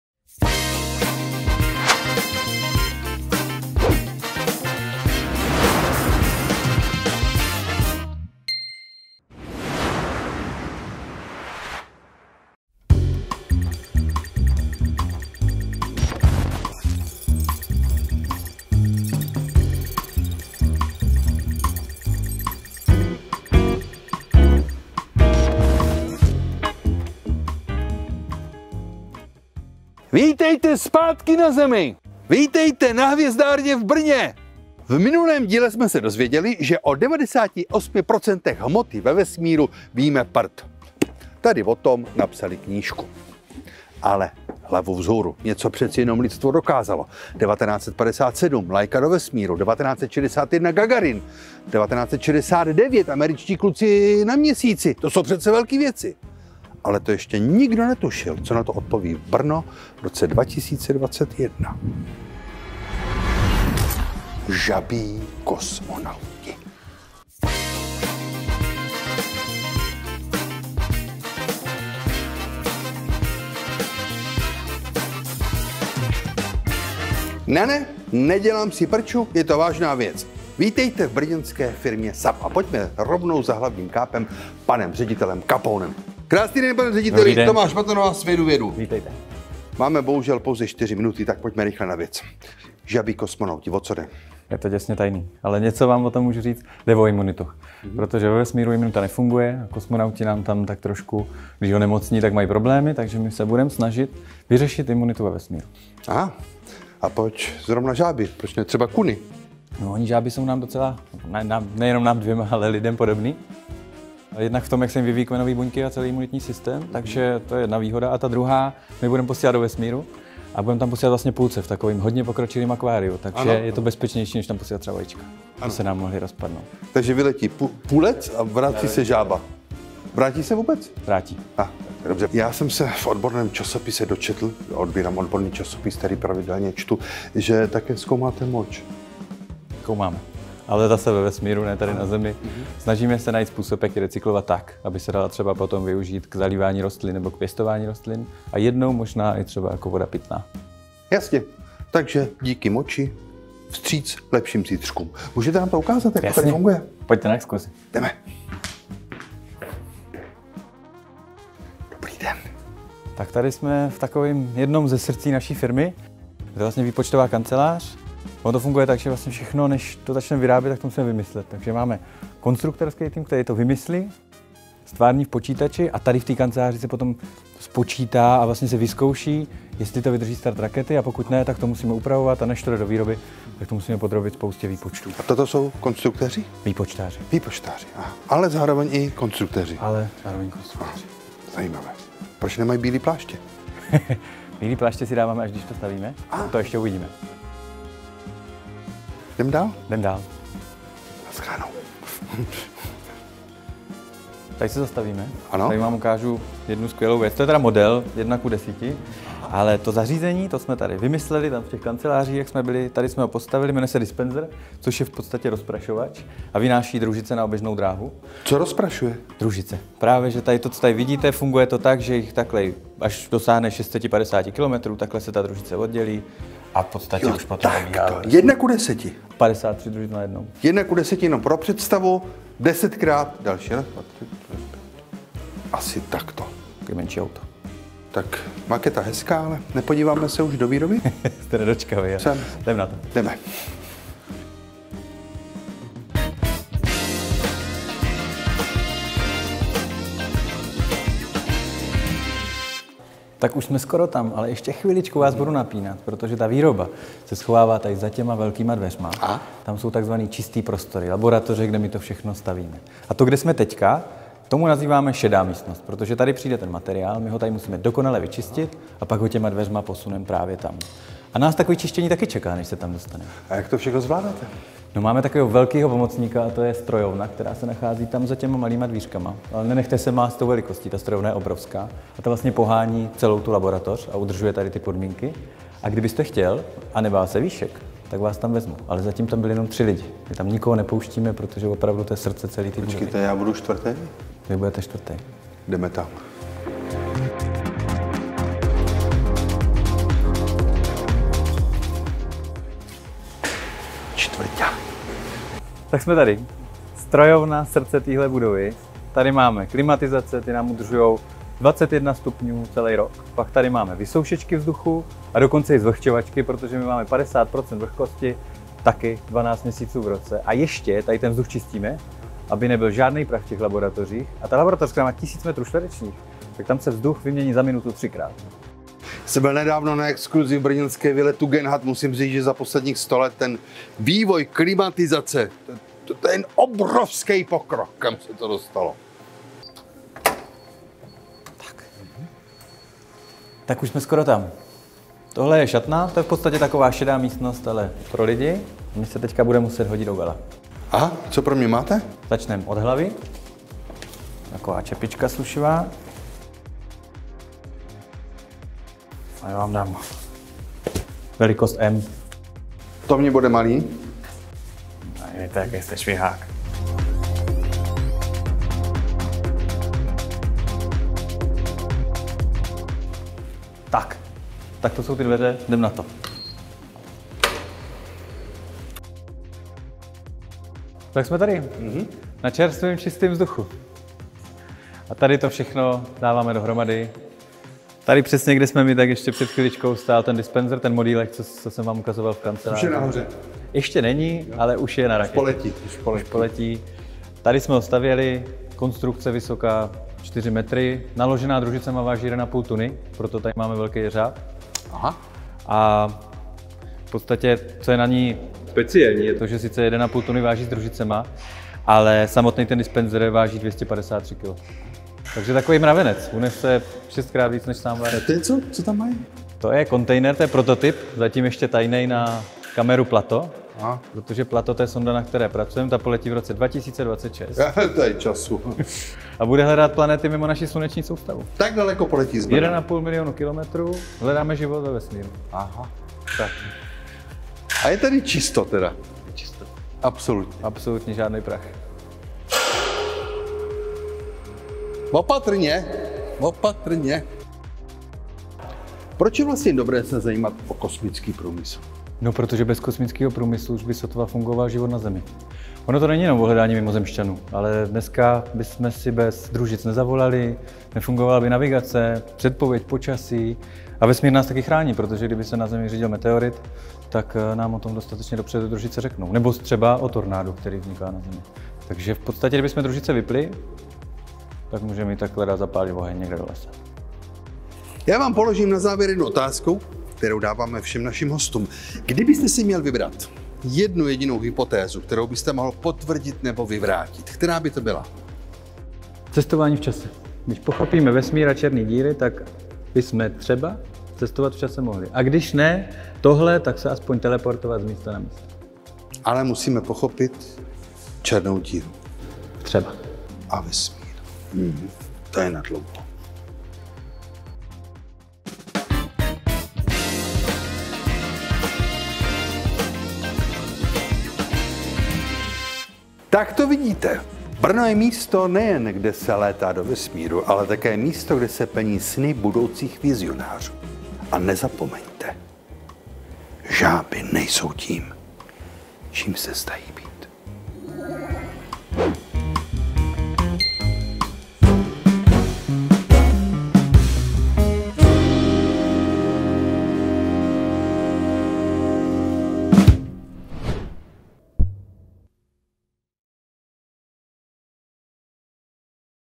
Svedu vědu | 02 | rakeťák Petr Kapoun z SAB Aerospace
Brno je úžasné místo pro vědu | přináší Hvězdárna a planetárium Brno